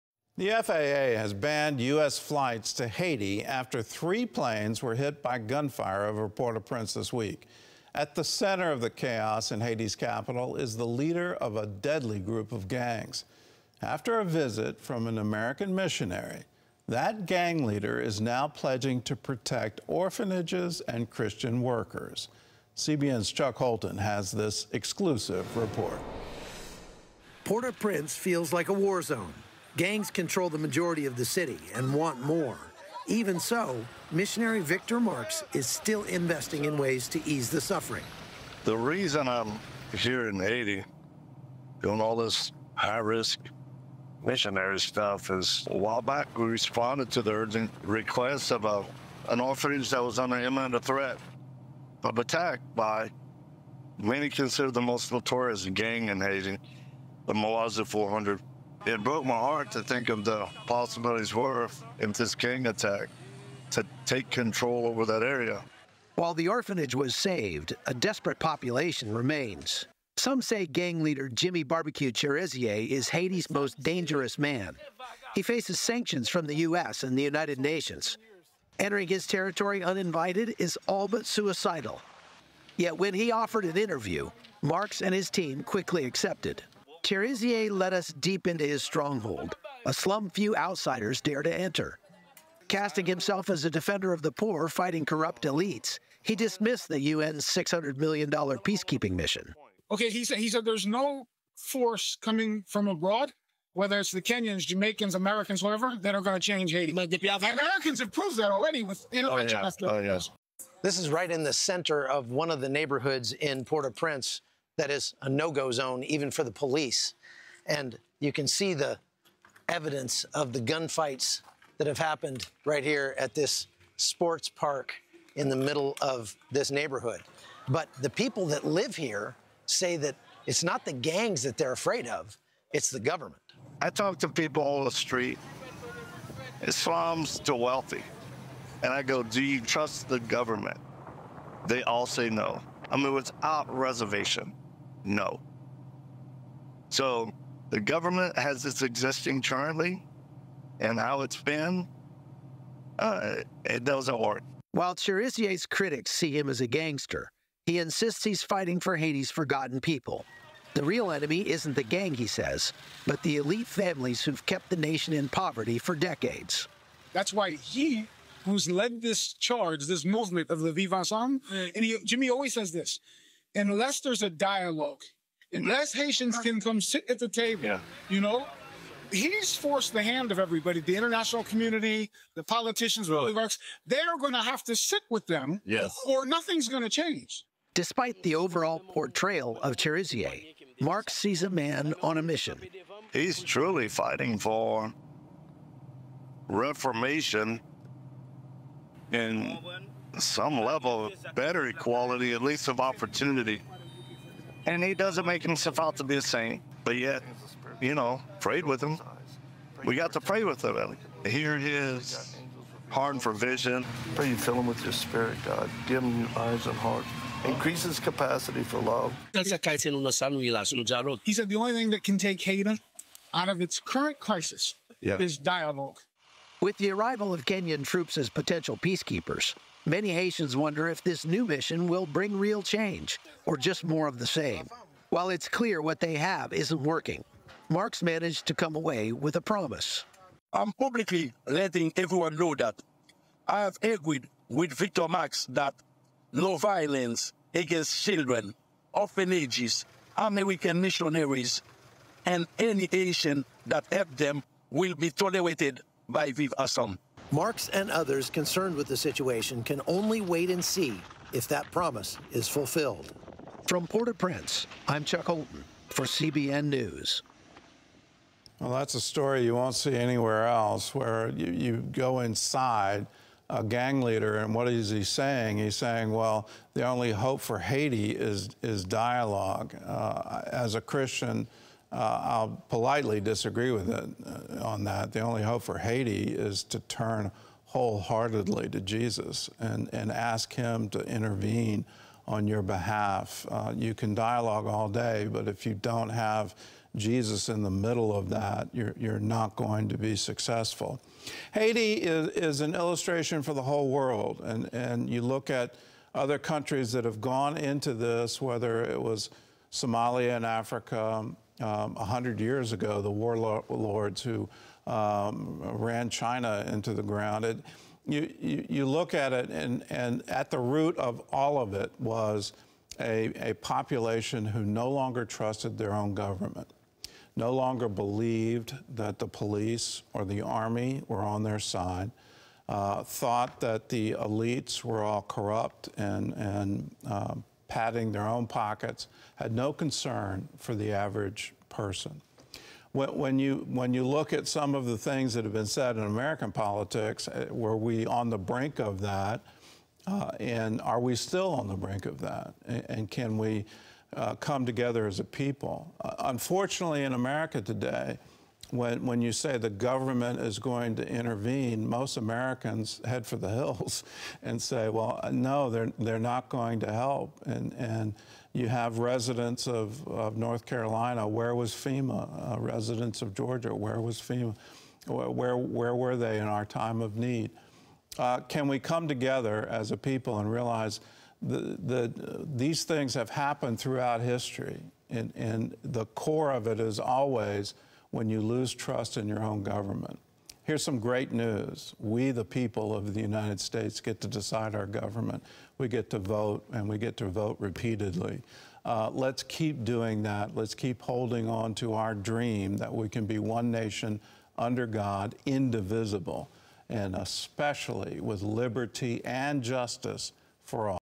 EXCLUSIVE: Victor Marx Speaks With Notorious Haitian Gang Leader Jimmy 'Barbecue' Chérizier
PORT-AU-PRINCE, Haiti – In the midst of escalating gang violence and political upheaval, Victor Marx of All Things Possible Ministries (ATP) recently traveled to Haiti, risking his safety to meet with some of the country's most notorious gang leaders. Known for his work in dangerous regions across the globe, Marx's mission was clear: negotiate for the protection of orphanages, missionaries, and children – the most vulnerable groups amid Haiti's spiraling unrest.

For months, the capital city of Port-au-Prince has been gripped by lawlessness as gangs tighten their control over the city, holding 85% of it under their sway. The political situation has deteriorated, with gunfire recently closing down Haiti's main airport and a new prime minister, businessman Alix Didier Fils-Aimé, being sworn in under highly contested circumstances.

Want more news from a Christian Perspective? Choose to support CBN